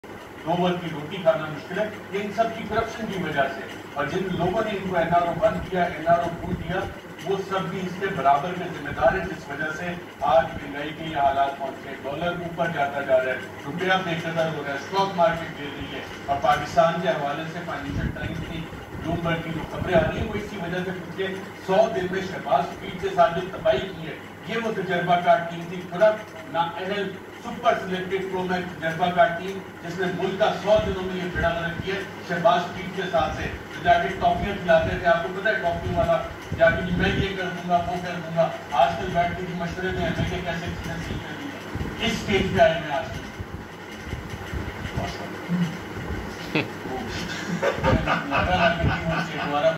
Funny Anchors in Pakistan - Kashif Abbasi Arshad Sharif - Shorts
Youtube Shorts:

See how anchor Kashif Abbasi made fun of Arshad Sharif in a live program.